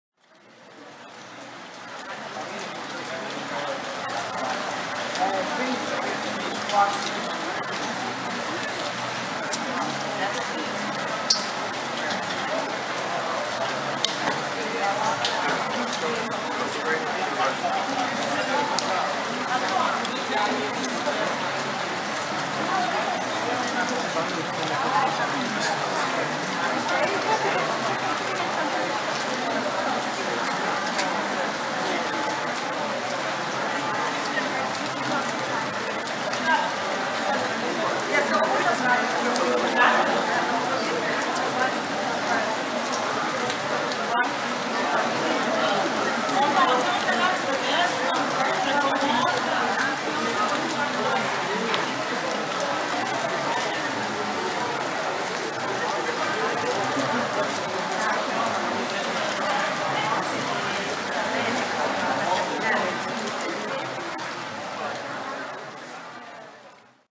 Venice Transit 05
Umbrella sales persons are doing a roaring trade